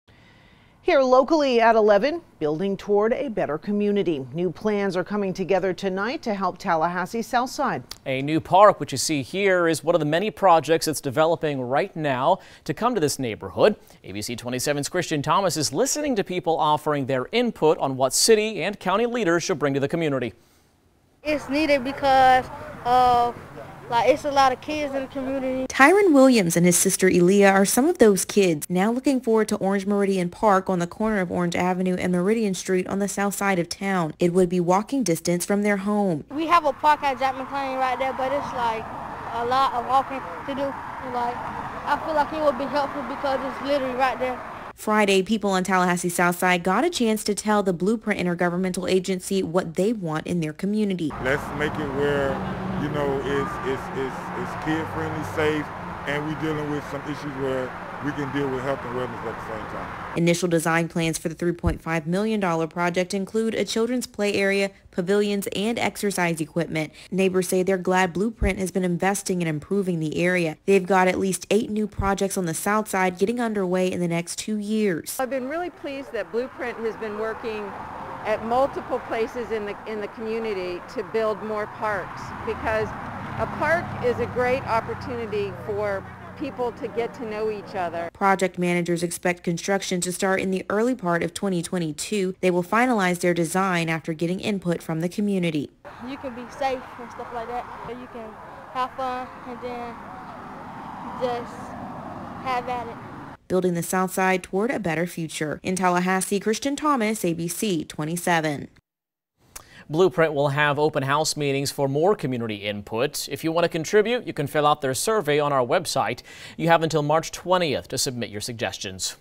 Southside residents weigh in on designs for new Orange-Meridian Park
The project has a $3.5 million budget and plans to start construction at the beginning of 2022